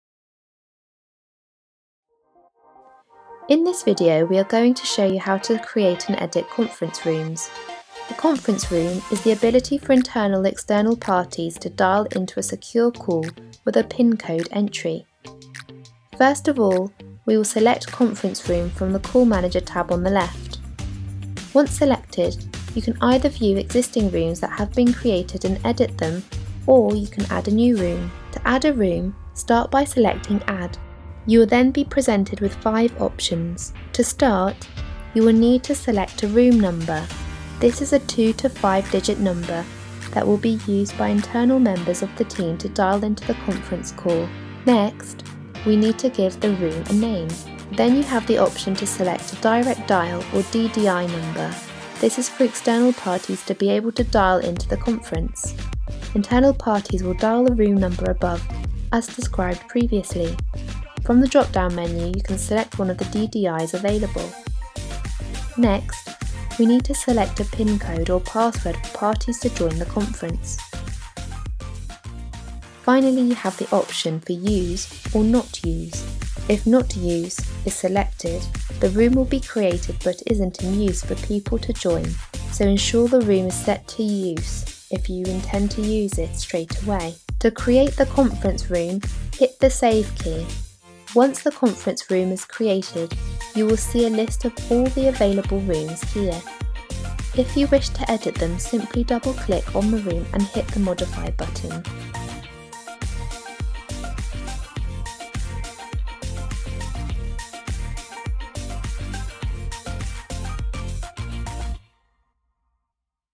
How to set up a conference room on the iPECS Cloud Portal
In this video we will tell you how to set up a conference room on the iPECS Cloud Portal.